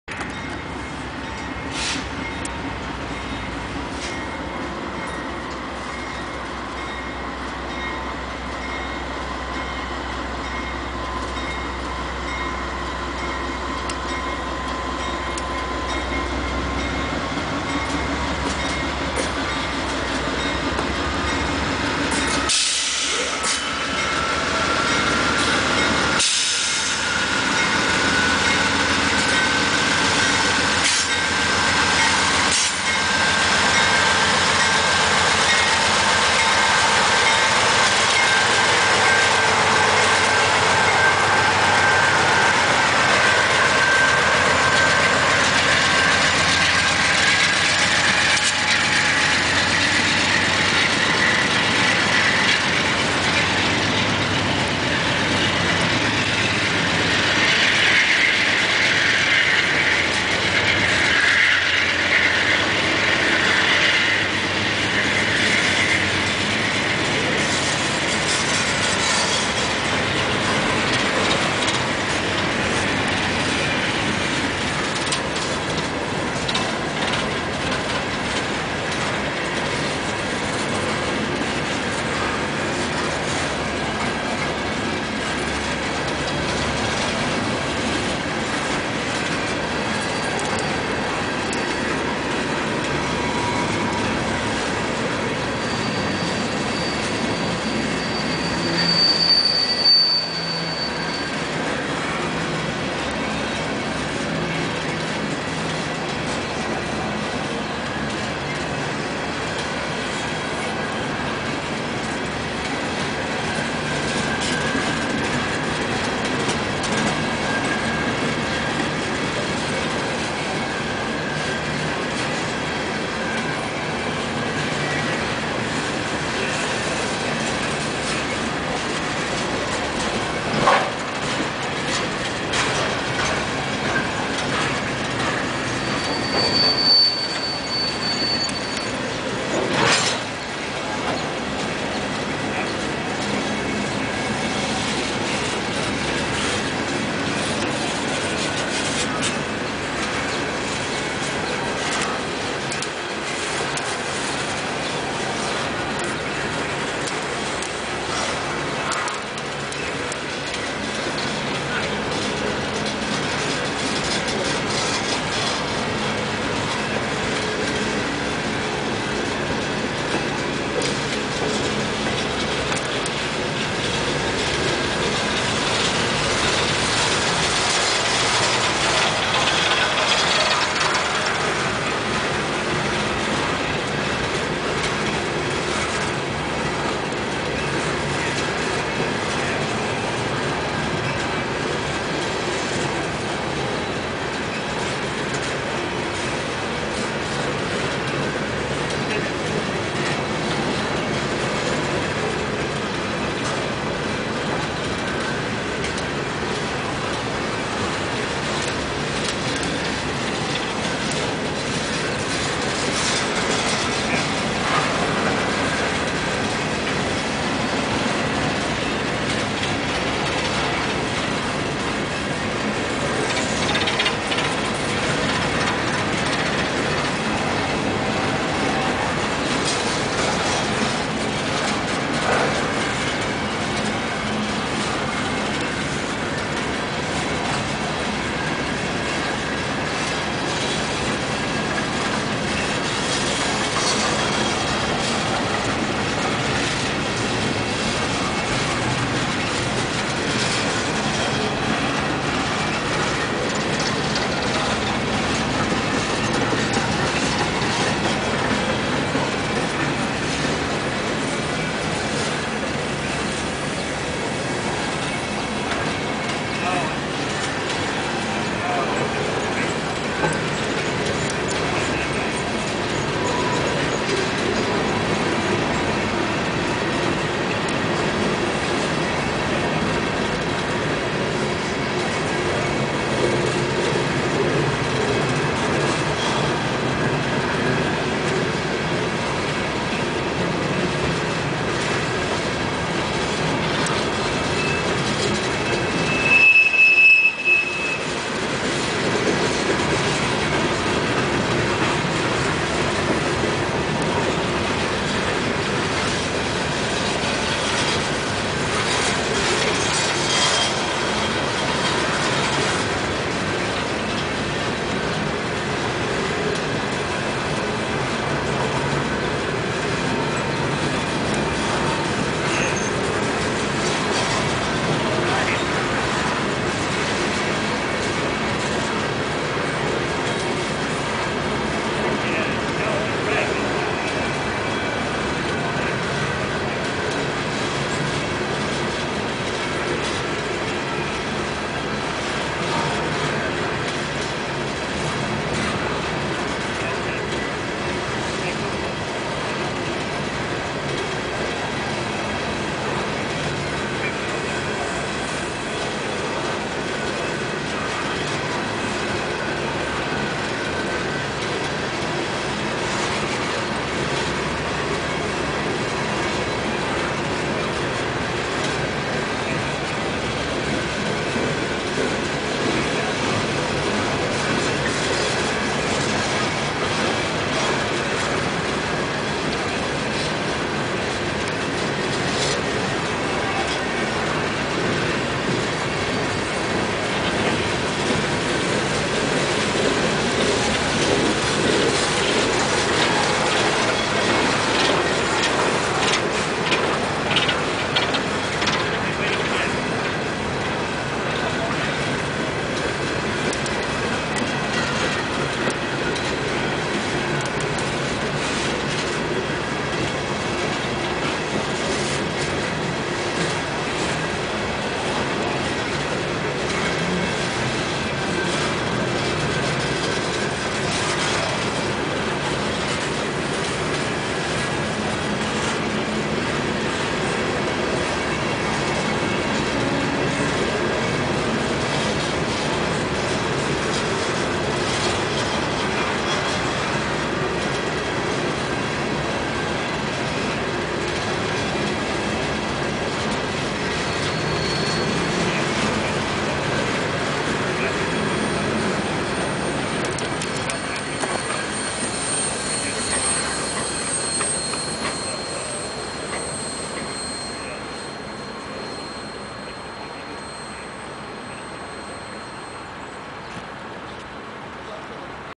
Csx coal train exiting the yard
ac44cw 364 and 249 leads a outbound csx coal train